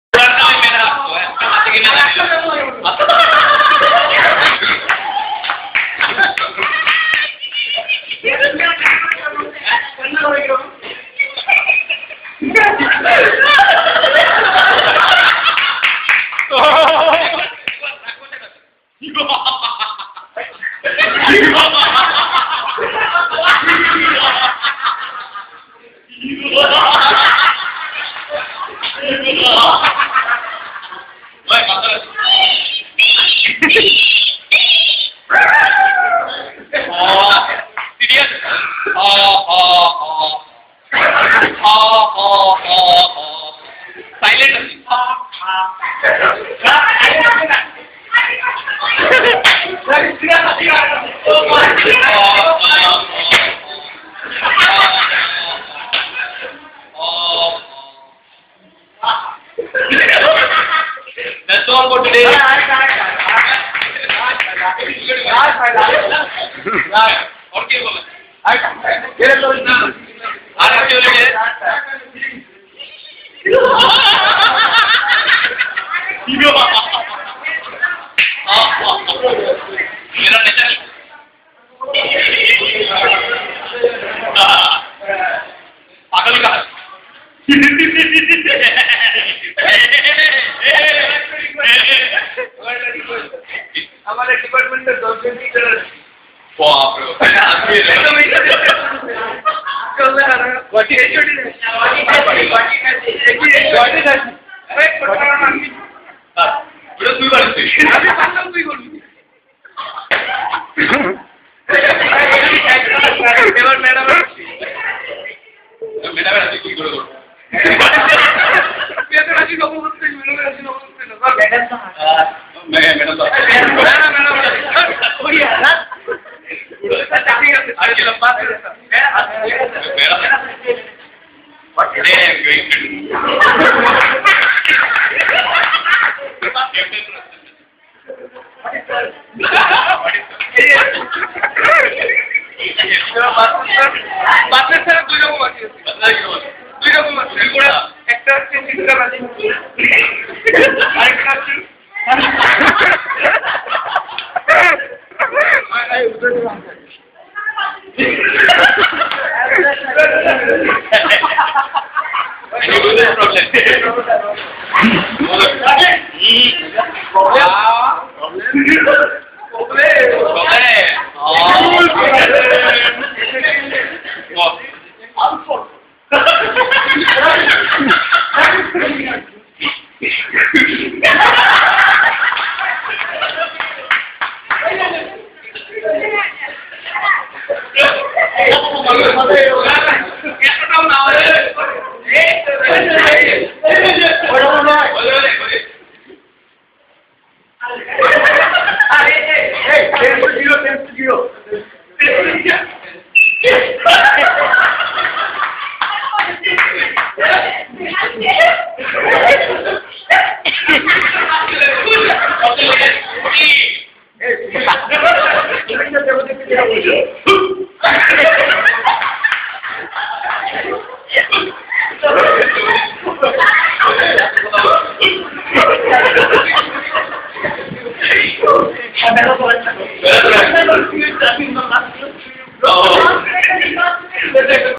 Laughter @ Debashish
Hello friends... if you are looking for something to cool your mind ... to relax your mood .. and to re-energize yourself then be assured that you clicked on the right video. Laugh aloud ... laugh bindaas ... laughter knows no bound !!! Go ahead and enjoy and do share with those who need some joy !! ... This is the "final Masti" of the Freshers 2010 celebrations by MSc Mathematics Students of our University in Silchar,  Assam, India ... Though the language spoken is Bengali , every people of the world can understand whats going on ... Because Laughter is a Universal Language !!! Yours laughingly Debashish Sharma :D